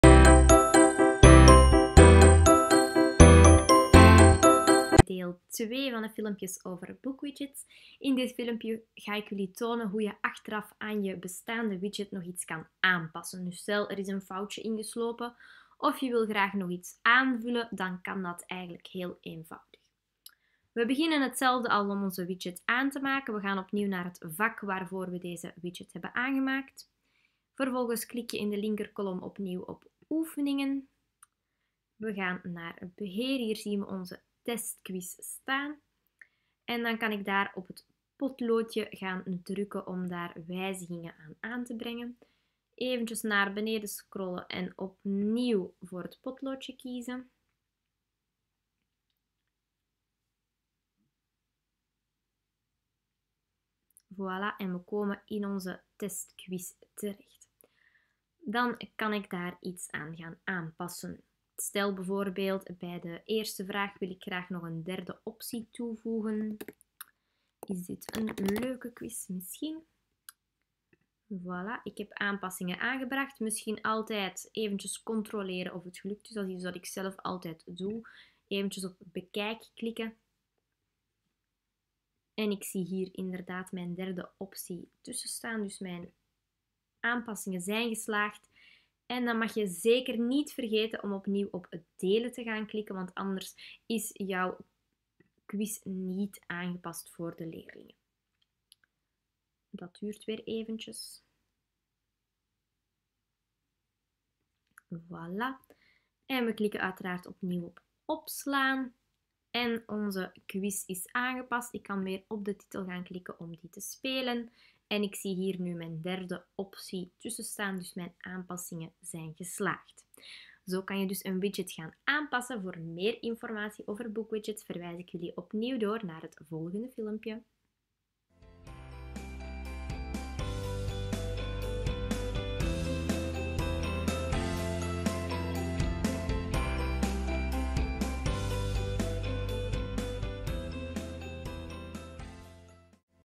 Instructiefilmpje 2 - bookwidgets aanpassingen doen
Aanpassingen doen in bookwidgets in Smartschool.